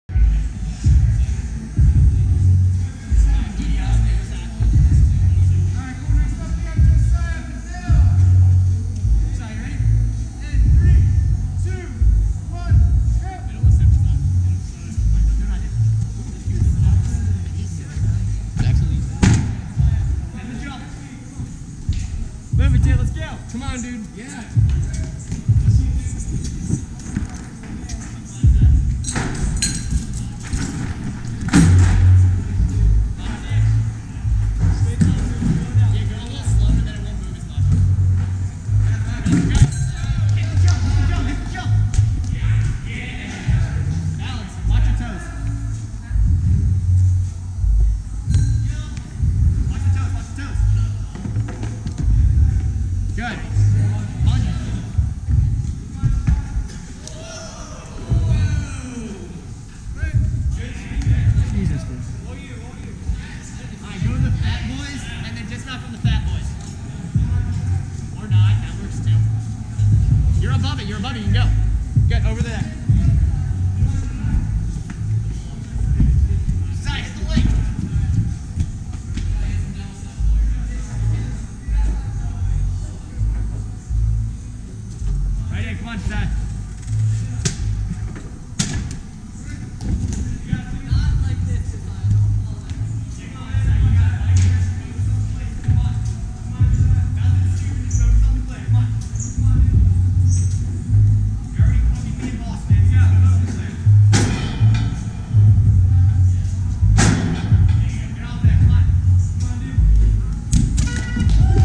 Josiah Pippel 1st Place iCore | National Ninja League Season 6
With five finishers, Josiah Pippel was right at home on this speed course, blazing his way onto the top of the podium.